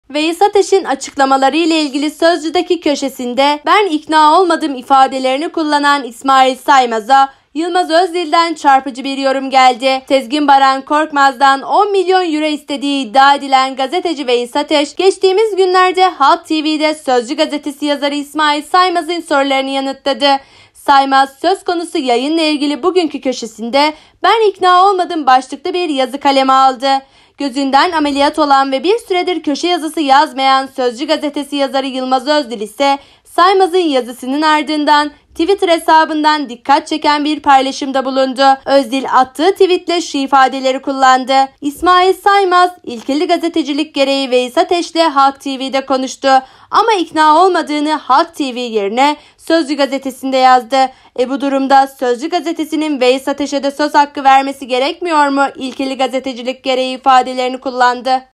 Yılmaz Özdil İsmail Saymaz'ı yerin dibine soktu
Sözcü gazetesi yazarı İsmail Saymaz, Sezgin Baran Korkmaz'dan 10 milyon Euro istediği belirtilen Veyis Ateş’in cuma günü kendisiyle yaptığı yayına ilişkin olarak, “Ben ikna olmadım” düşüncesini dile getirdi.